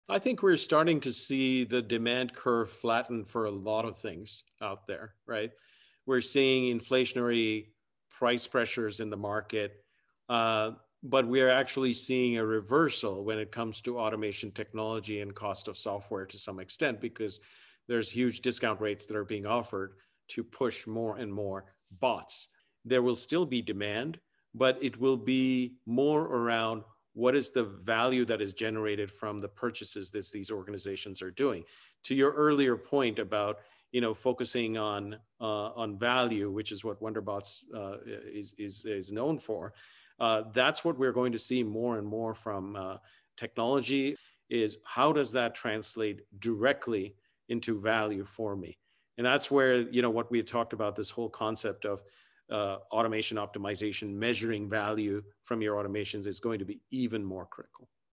In an economic slowdown, will we see an increased push towards Digital Workers?
In the current state of the world, will automation adoption be faster and bigger, or will it take a step back? Hear what Shail Khiyara, Founder of VOCAL has to say about the future of this trending technology.